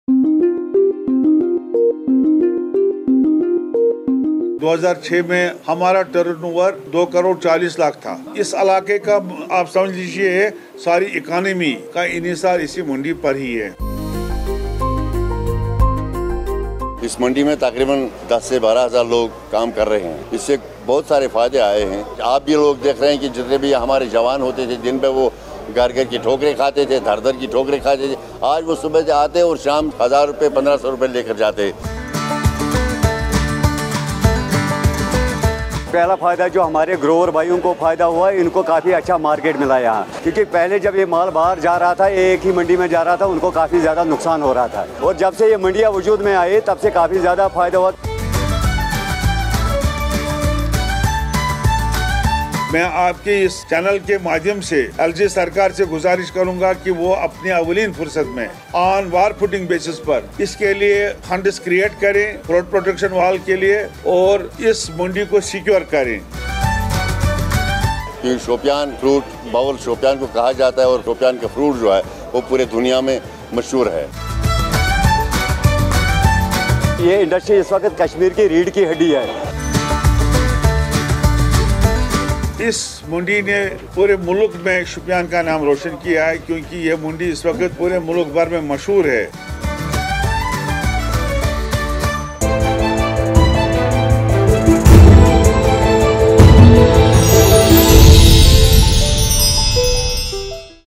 Shopian economy depends on its fruit mandi, but the mandi itself lacks a protection wall
In 2006 our turnover was Rs. 2 crore and 40 lakh says Mohd Ameen Peer, President Fruit Association, Shopian. The economy of the whole area depends on this fruit mandi at Shopian. Around 10-12,000 people work in this mandi and we are getting a lot of benefits says Peerzada Shabir Ahmad, President Pesticide & Dealer’s Association, Shopian. The youth who were wasting away their life now work in this mandi from morning till evening and take home around Rs 1000-1,500 per day. Many apple fruit growers have benefitted as they have got a good market in this mandi. A fruit grower Shamim Ahmad says, earlier, the fruits went out to only one mandi because of which their business was hurting. After the J&K Department of Horticulture, Planning & Marketing set up various mandis, the apple fruit growers are experiencing a lot of benefits. The fruit industry is Kashmir’s backbone. President Pesticide & Dealer’s Association appealed to the LG administration in J&K to create a protection wall on a war footing around the mandi and secure it. Shopian apple fruits are famous the world over and the Shopian mandi is famous in the whole country.